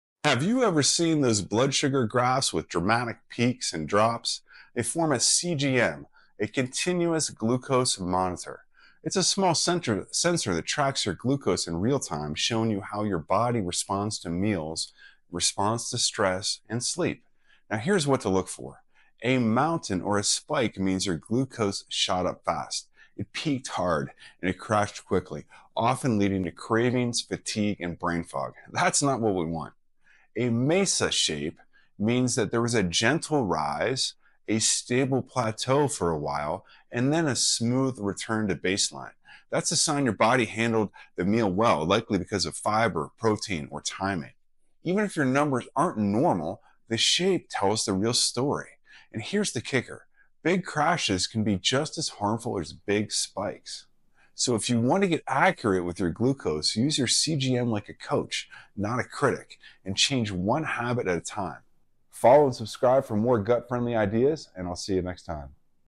How to Read CGM Curves Like a Pro!
A CGM (continuous glucose monitor) shows your real-time glucose curves—spikes, crashes, and patterns that matter.Here’s how to read the shape: mountain vs. mesa.
📊 Glucose patterns decoded🧠 Gut, meals & stress tracking#CGMexplained